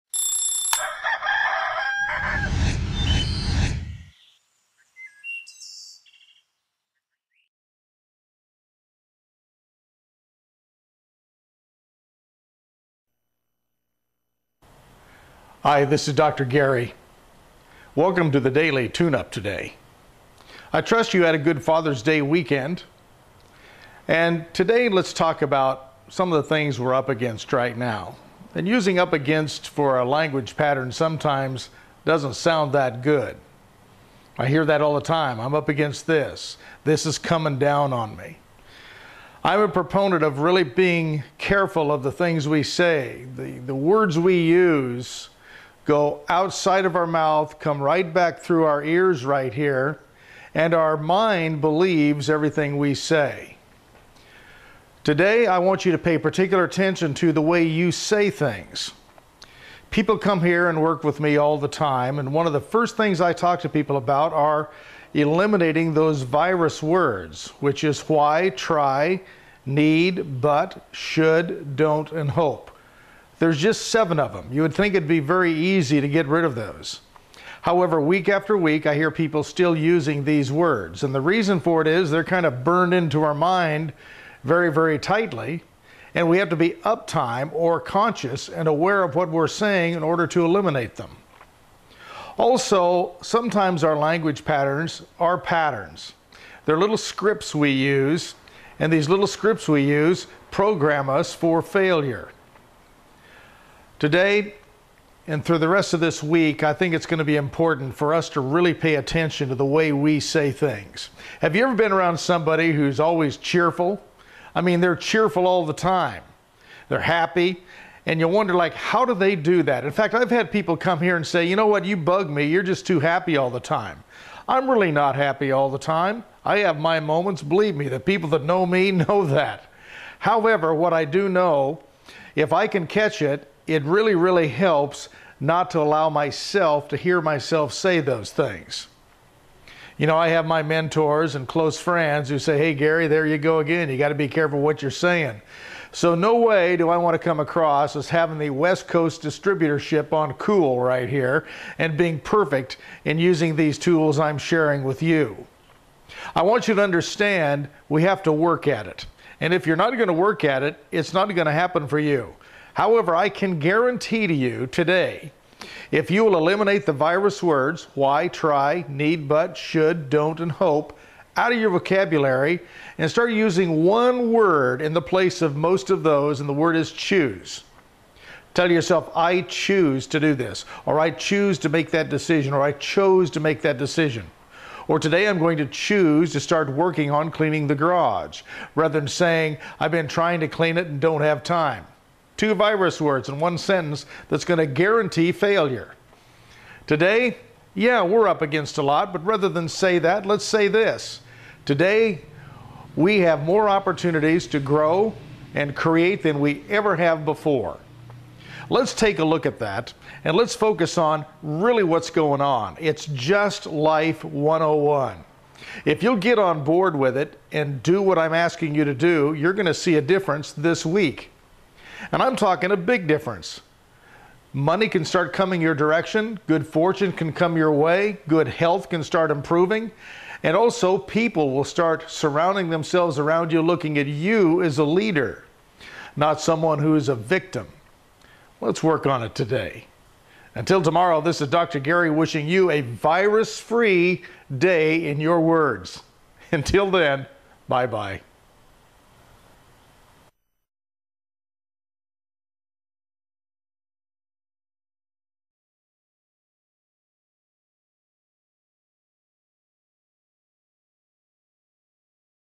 Your Mind Believes What You Say
We are constantly programing our future by the way we talk to ourself